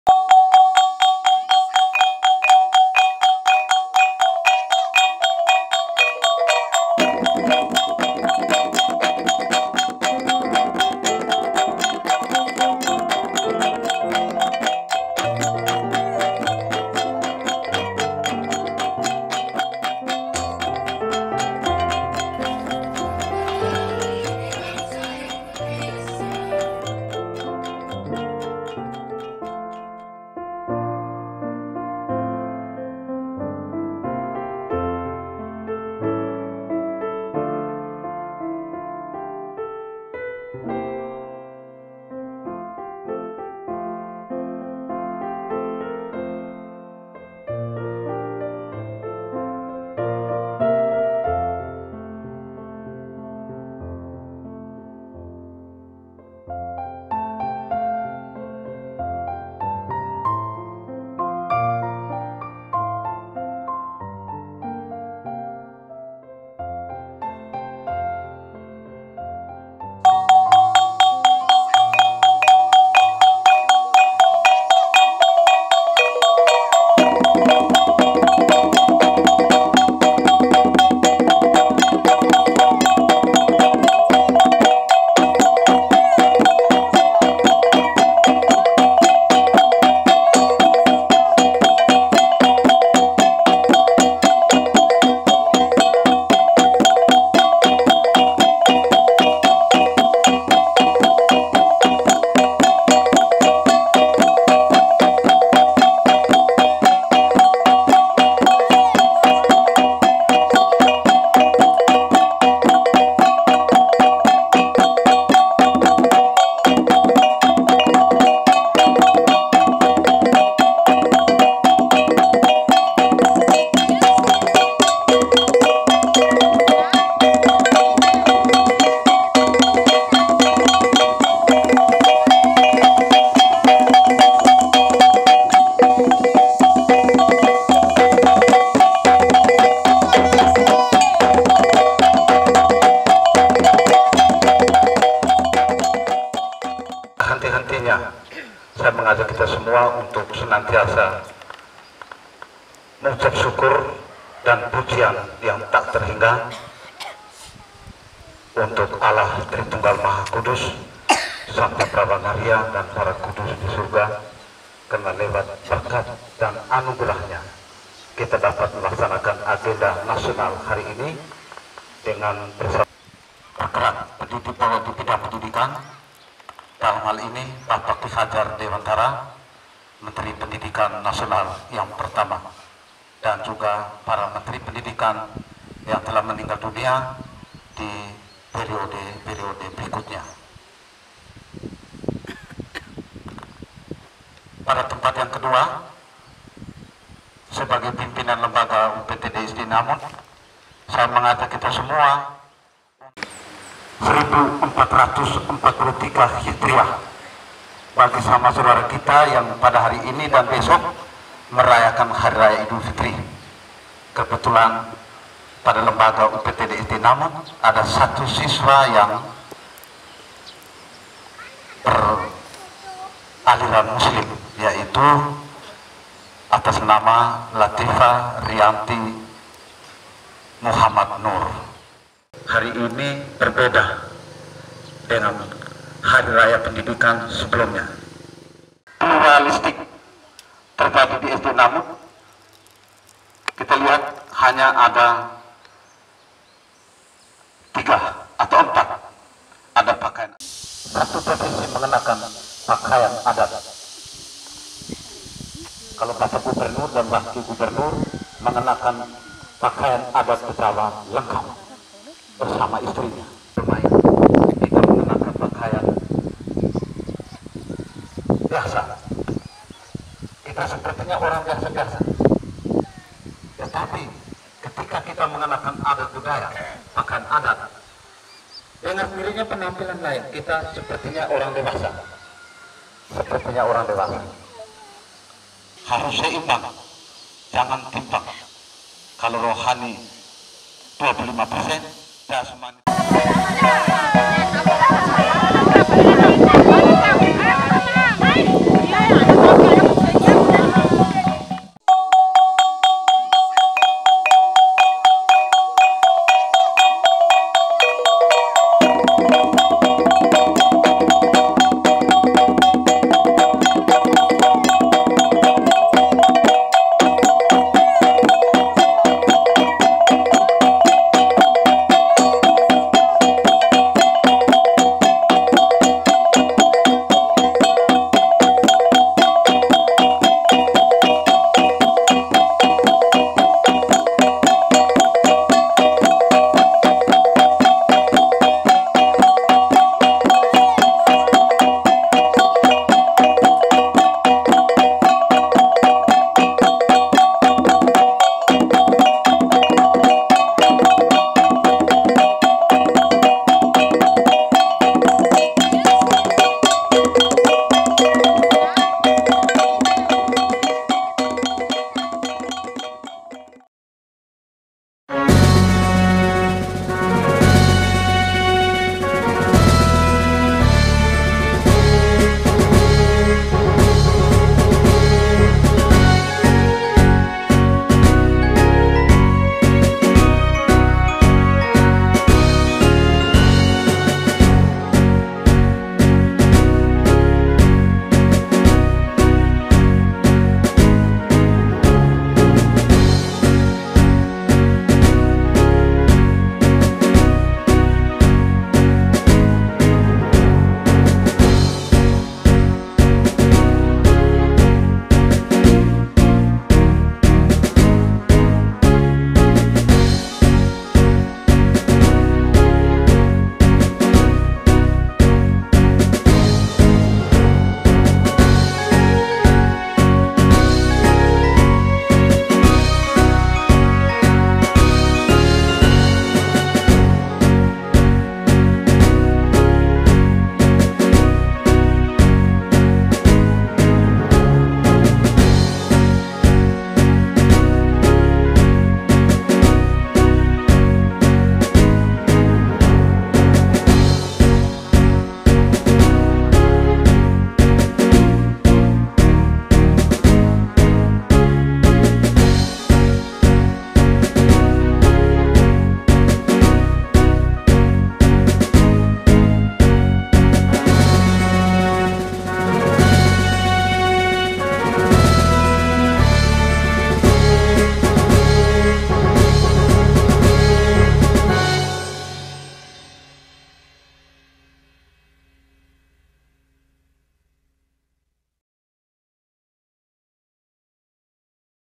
UNIK !!! Upacara Memperingati HARDIKNAS 2 Mei tahun 2022 // Pesrta Berbusana ADAT ( SDI Namut )
UNIK !!! Upacara Memperingati HARDIKNAS 2 Mei tahun 2022 // Pesrta Berbusana ADAT ( SDI Namut )UNIK !!! Upacara Memperingati HARDIKNAS 2 Mei tahun 2022 // Peserta Berbusana ADAT ( SDI Namut )

Upacara Memperingati HARDIKNAS 2 Mei 2022 berlokasi di UPTD SDI Namut, Desa WOlomeze Dua, Kecamatan Riung Barat, Kabupaten Ngada, Provinsi Nusa Tenggara Timur.
Pada Upacara Kali ini sangatlah unik tidak seperti tahun tahun yang sudah berlalu. Menariknya, saat upacara berlangsung, semua peserta berbusana ADAT Tradisional dan pada saat Mengheningkan Cipta dikala para peserta menundukan kepala, diiringi bunyian musik tradisional Namut ( Wolomeze Dua) Yakni Gong Gendang ( Nggo Lamba ) yang dimainkan oleh para peserta didik UPTD SDI Namut. Selain Para Dewan Guru dan Peserta Didik, turut hadir juga Orangtua Peserta didik dan Tokoh Adat setempat.

Terimkasih telah berkunjung. Salam...

T    E      R     Z     O     N           N    G    A    Z    O         P   A   P    A         A     X    E     L
Terzon Ngazo
Namut Punya Musik